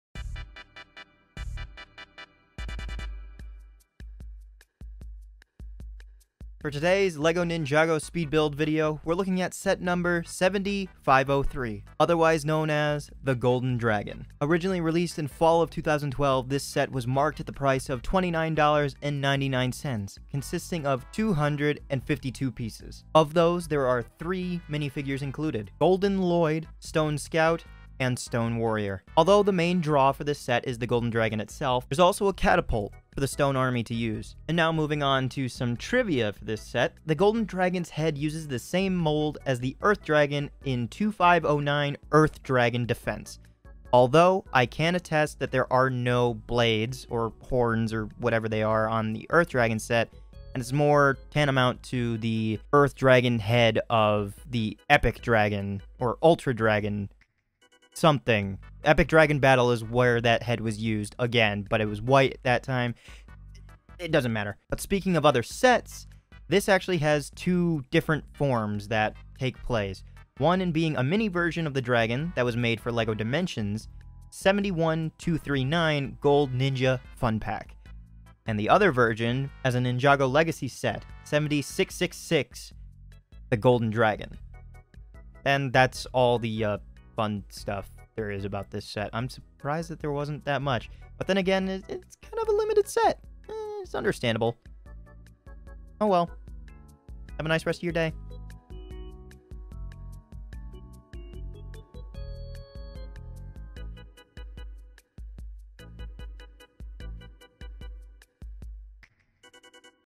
Lego Ninjago 70503 The Golden Dragon
Today's Lego Ninjago speed build video is dedicated to set 70503 The Golden Dragon. The set not only comes with the Golden Dragon itself but also the Gold Ninja... Golden Lloyd...

Appreciative of any likes, comments, or subscriptions!